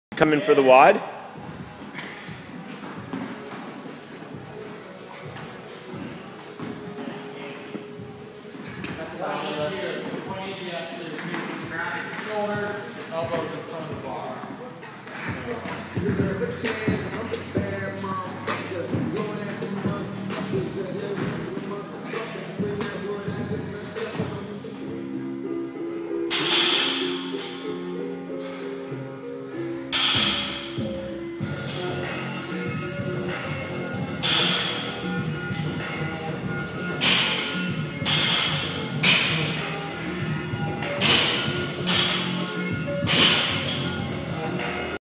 Run - SDHP and burpees
4RFT:
25 bar facing burpees
800m run
15 SDHP 95/65